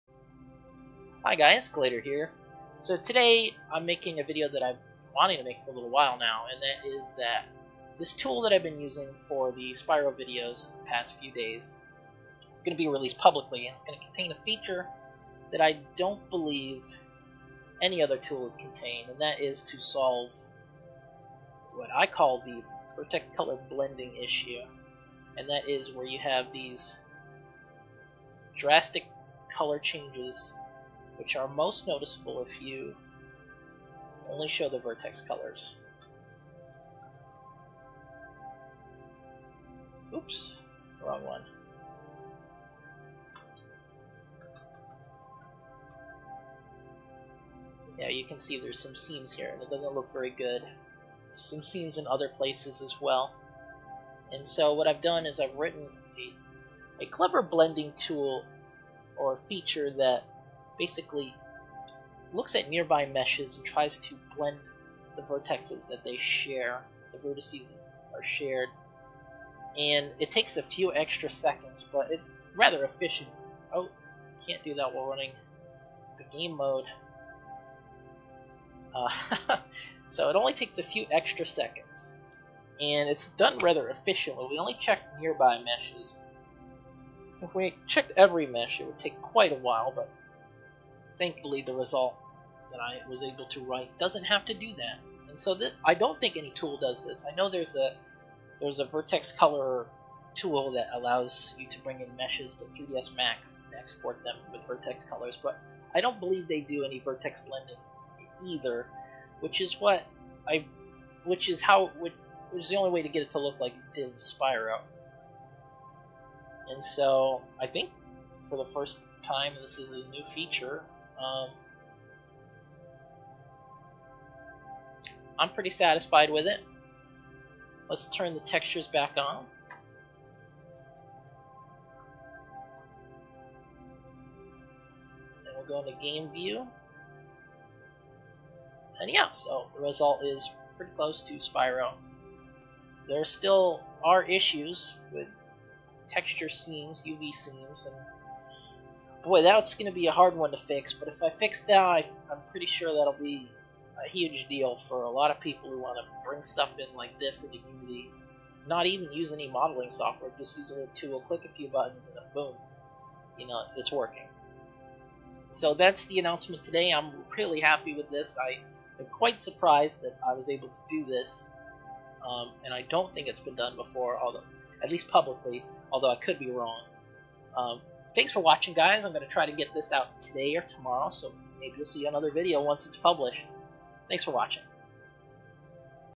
Spyro Unity3D - Vertex Color Blending (Episode 4)
Wrote a shader and tool to render vertex colored and lightmapped Spyro maps in Unity3D. It's an incomplete tool and doesn't produce perfect results yet. There are also unsolved UV issues coming in from Spyro World Viewer. Not sure how to automate fixing this yet.

This tool now also supports the concept of vertex color blending at edges where it connects to other meshes. This tool will be free, opensource and available on my Github soon.